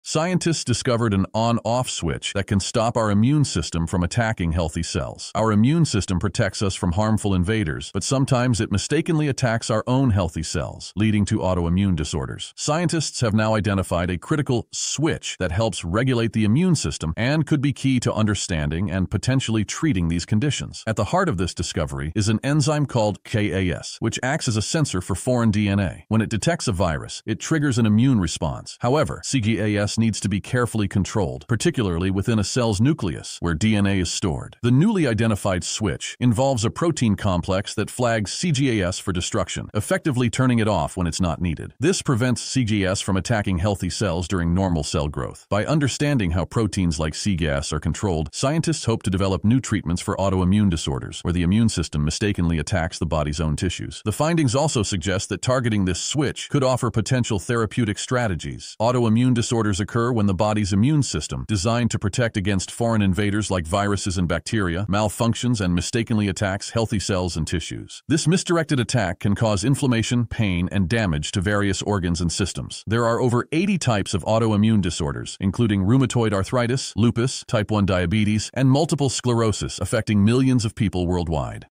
Autoimmune Diseases on Notice - Scientists Find the Key to Stopping Them!
Big win for immunity! Researchers discover a mechanism that prevents the immune system from attacking healthy cells, offering hope for future autoimmune disease treatments.

Researchers recently discovered a mechanism acting like a switch that prevents the immune system from attacking our healthy cells. This is a significant finding because it could lead to new treatments for autoimmune diseases.

Here's a simplified breakdown:

* Our immune system protects us from harmful invaders like viruses and bacteria.
* A protein called cGAS acts like a lookout, detecting foreign DNA and triggering an immune response.
* But if cGAS stays active for too long, it can mistakenly attack healthy tissue.
* Scientists found that cells use a process called ubiquitination to tag cGAS for destruction, essentially turning off the switch and preventing this mistaken attack.

This discovery improves our understanding of how the immune system avoids attacking itself. It paves the way for potential therapies that regulate this process in diseases where the immune system malfunctions.